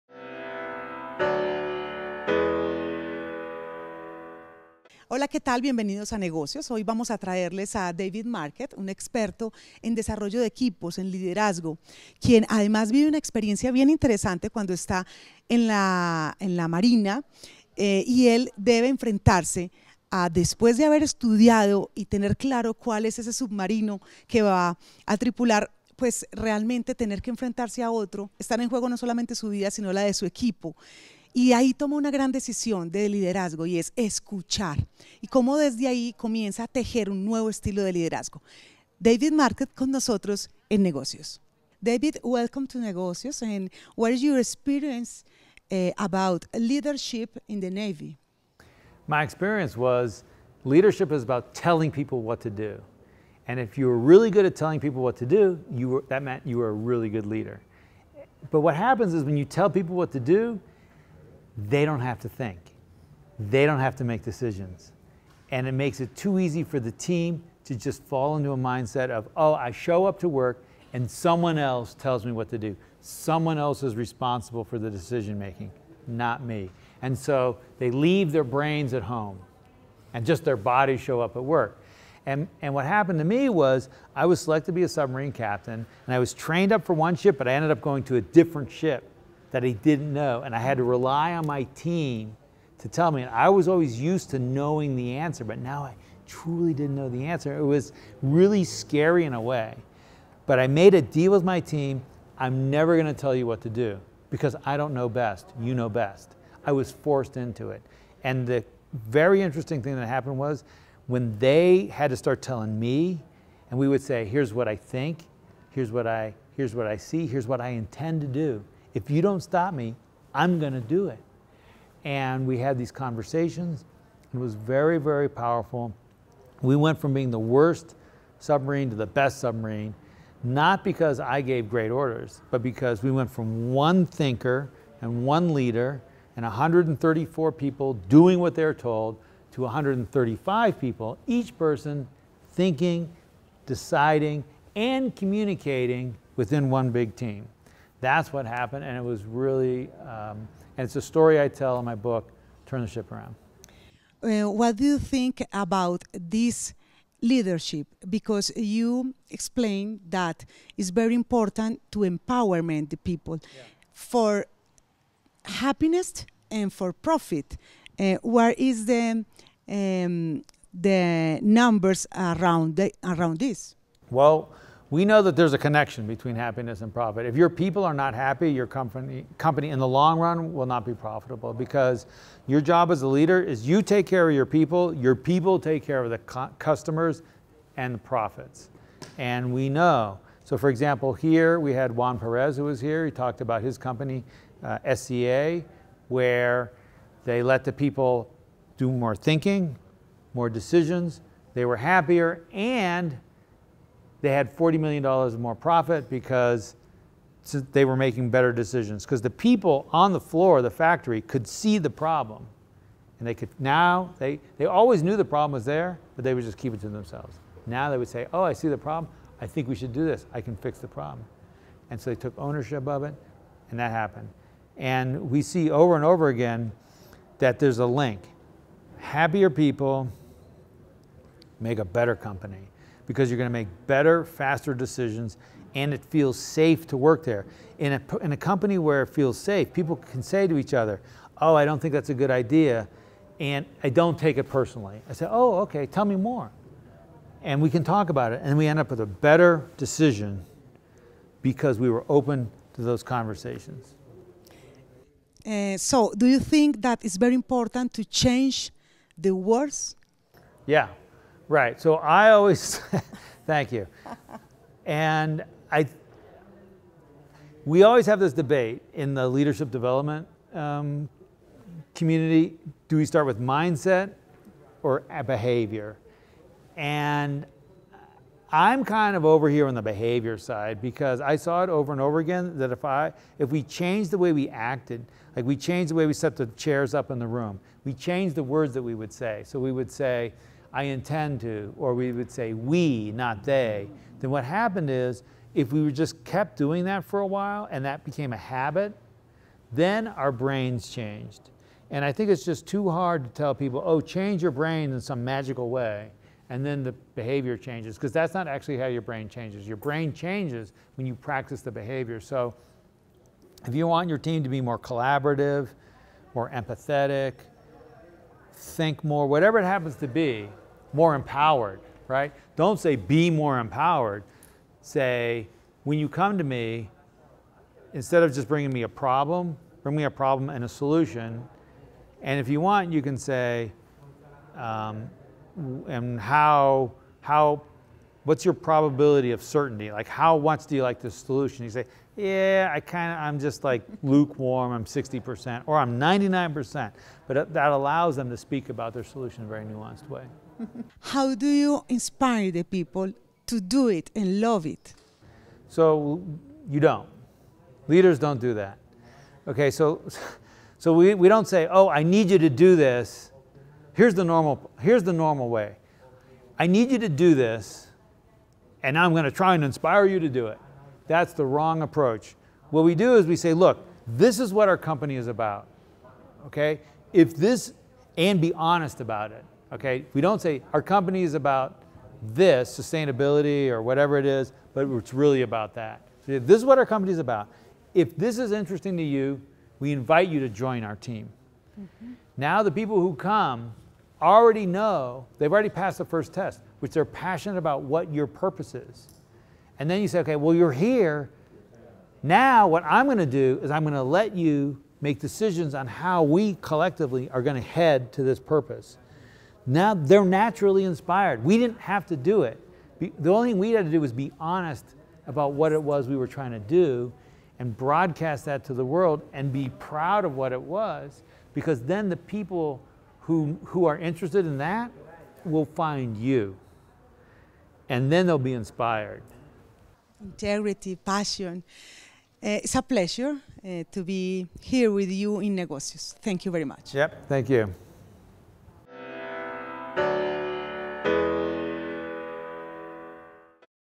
Escuchar para empoderar, mejorar rentabilidad y felicidad - [Negocios en Tm] ®
La empresaria y coach Internacional Paola Rueda López (@paolaruedalopez) conversó con David Marquet, experto internacional en Liderazgo, sobre la importancia de empoderar a los empleados, brindándoles felicidad y mejorando la rentabilidad de la empresa.